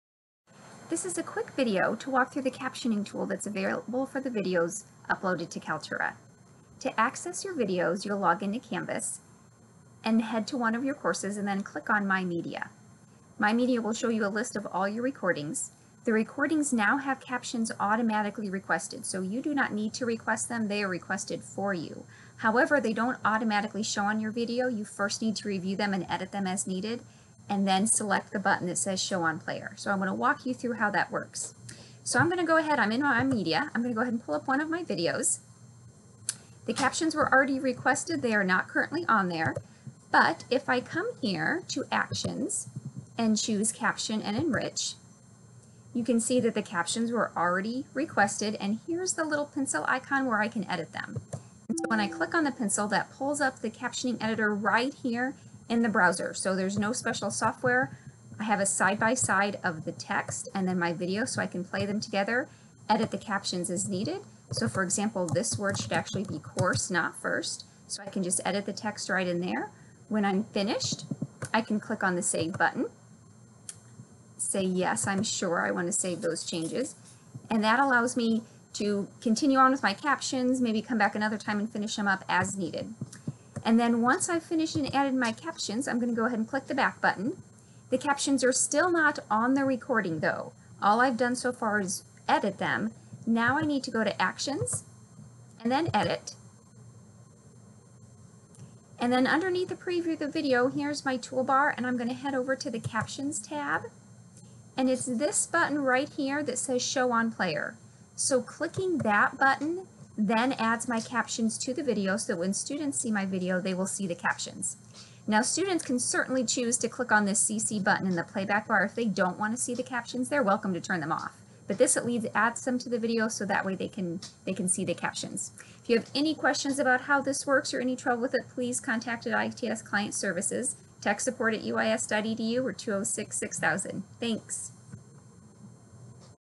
Viewing and Editing Captions in Kaltura Media
In this video, you will learn how to edit captions in Kaltura Media and show them on the player in Canvas.